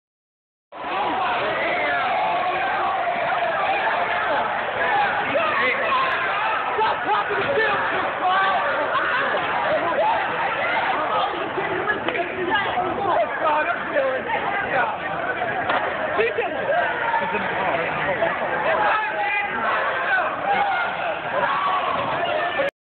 RCW Womens Wrestling Haze Yim Ray
RCW invading The Du Burns Arena in Baltimore Maryland showcasing top notch womens wrestling talent. Featuring Daizee Haze, Mia Yim and Sara Del Ray.